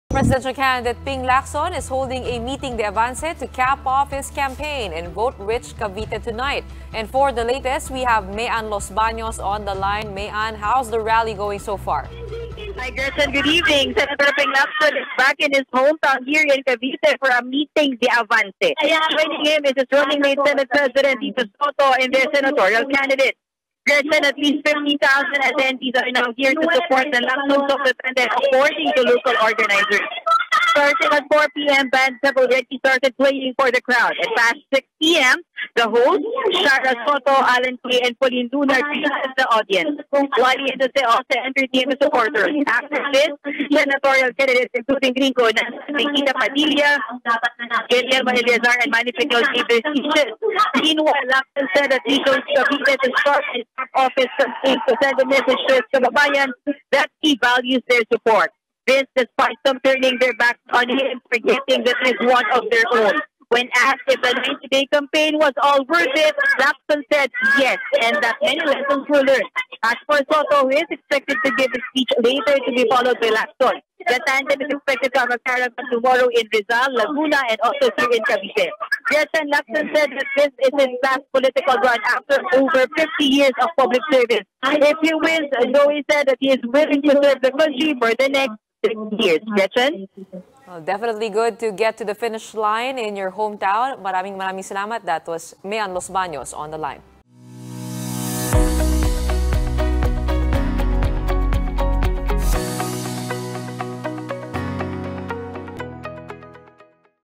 Lacson returns to home province of Cavite
Presidential candidate Sen. Ping Lacson returns to his home province of Cavite for a miting de avance, saying he values the residents' support. He also maintains this is his last political run.